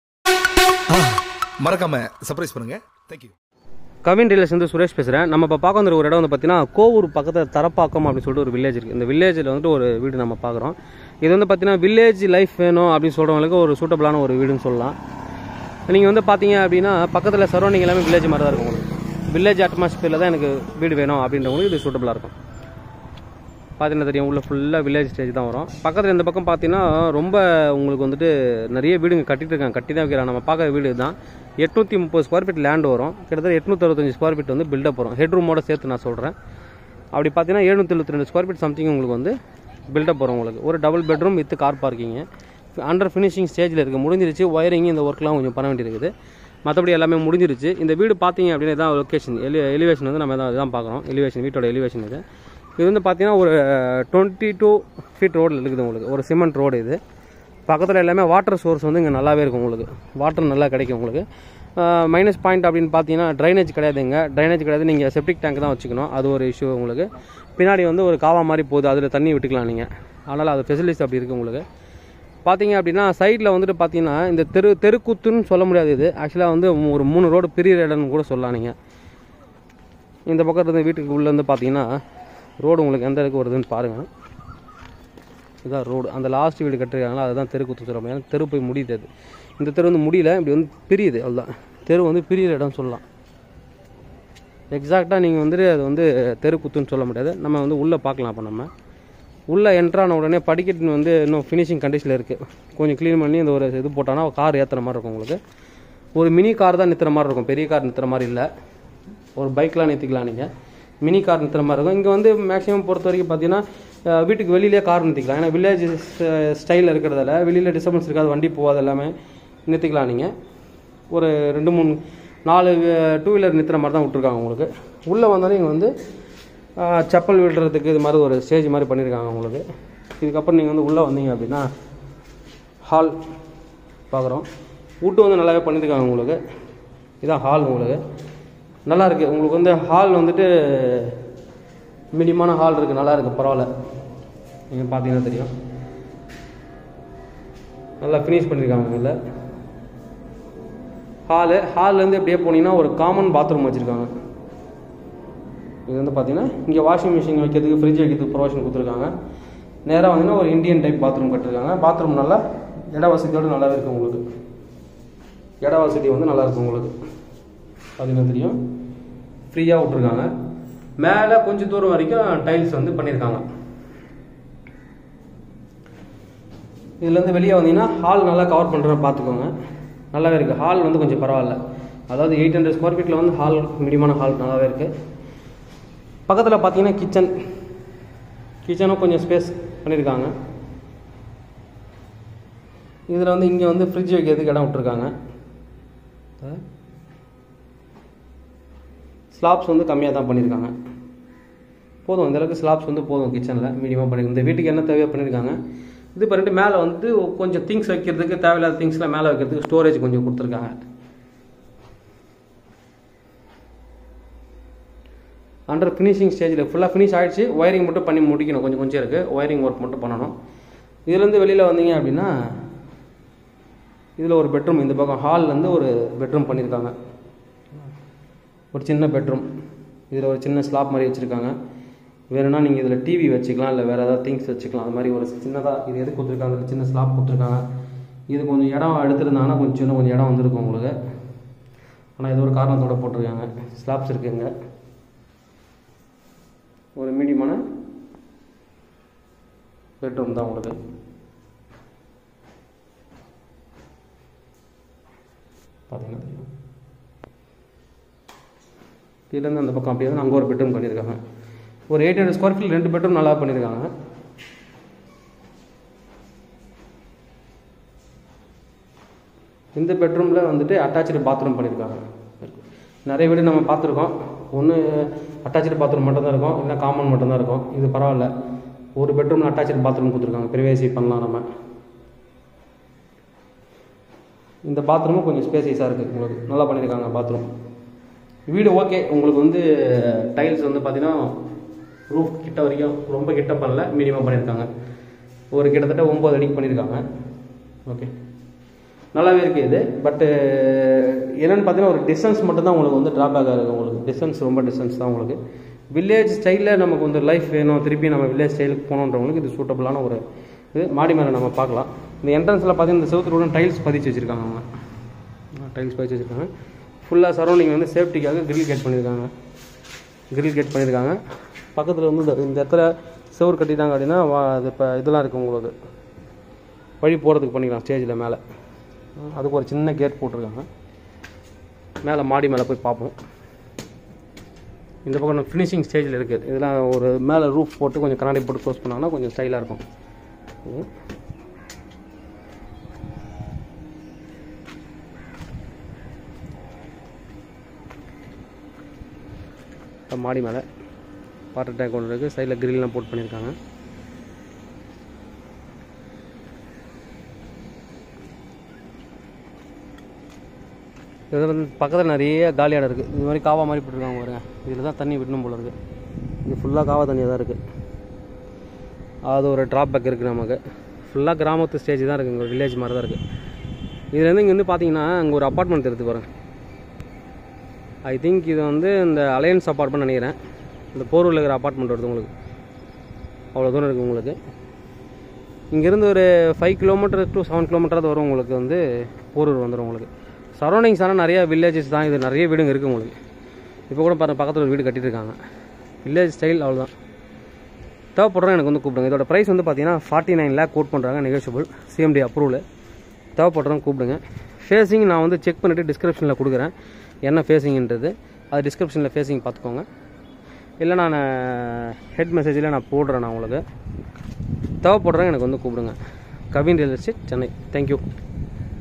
ID 391- Individual house sale ||New 2BHK||Near Porur||49L
New Individual house Sale in Girugambakam*
 Ready to occupy  individual House
Land 837sqft
Build up 772 sqft
2bhk
CCP
South facing Land
7.5Km From Porur
East facing house
Cmda approved
Price 49L negotiable